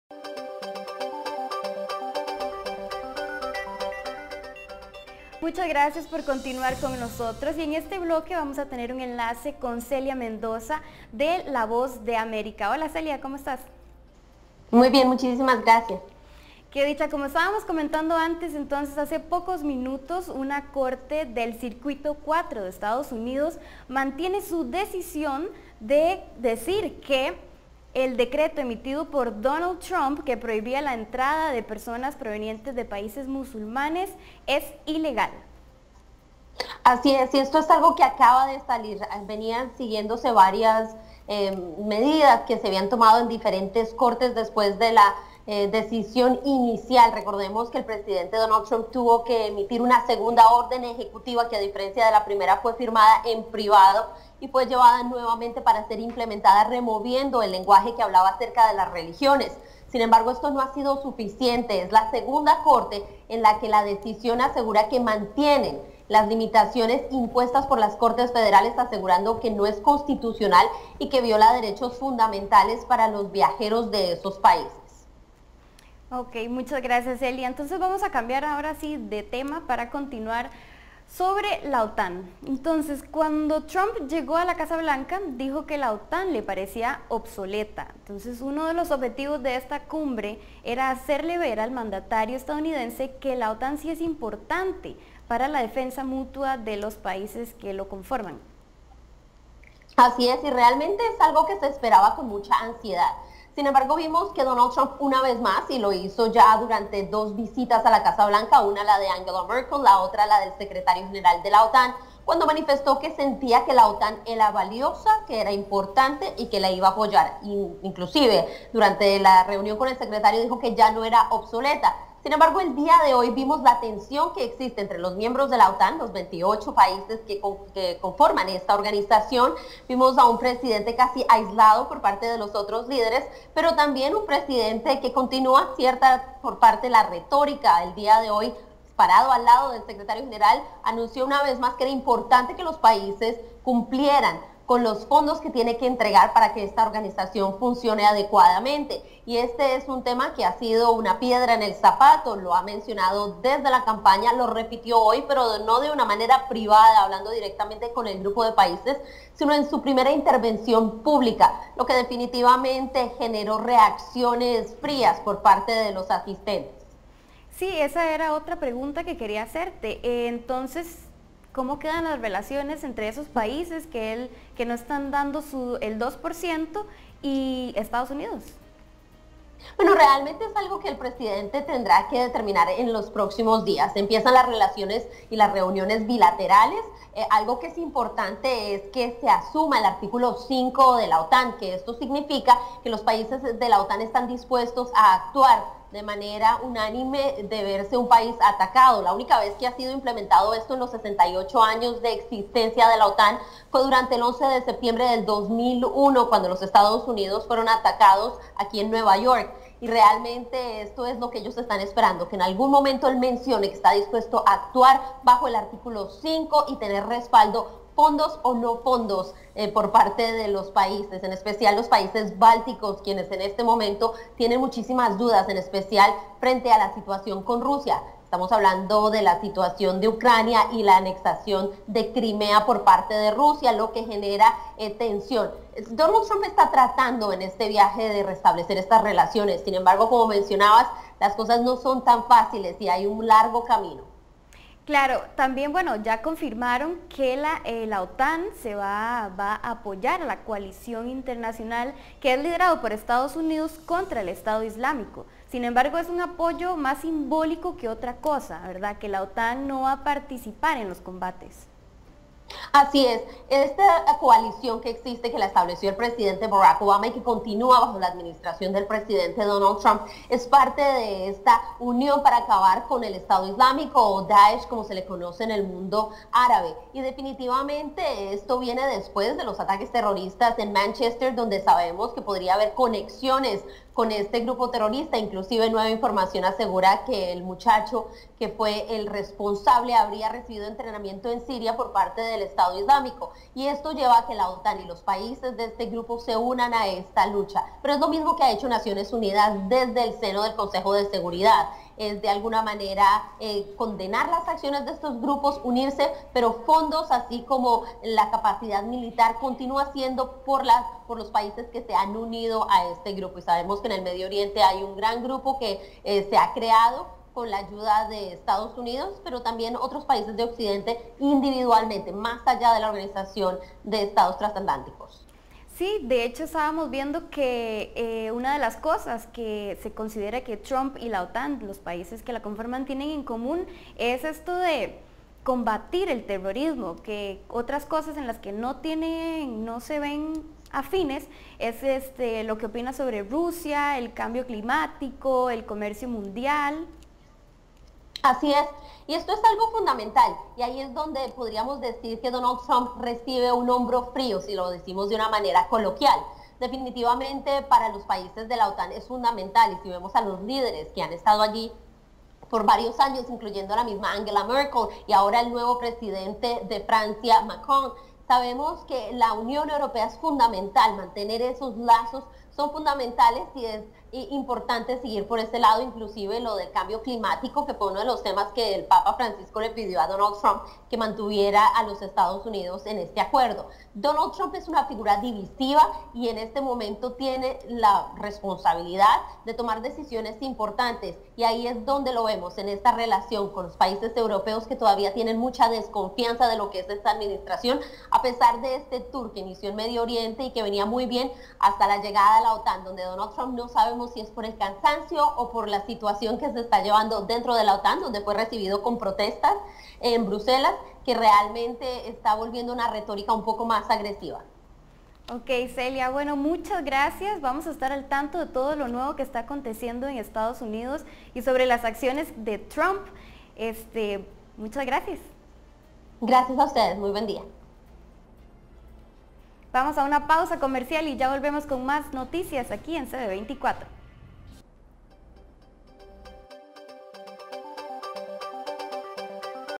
Presidente de EE.UU. Donald Trump participa en su primera Cumbre de la OTAN
Para ampliar sobre la primera Cumbre en la OTAN del Presidente Donald Trump, conversamos con Celia Mendoza de la Voz de América.

Redacción / CB24